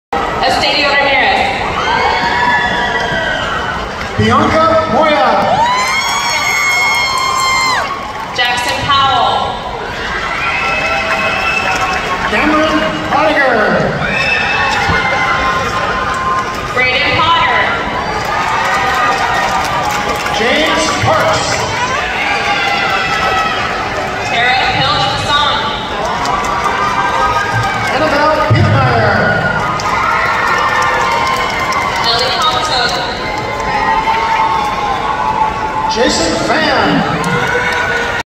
Bianca's Highschool Graduation Day
Bianca's Highschool Graduation Day at Angels of the winds Stadium in Everett, WashingtonJune 18, 2022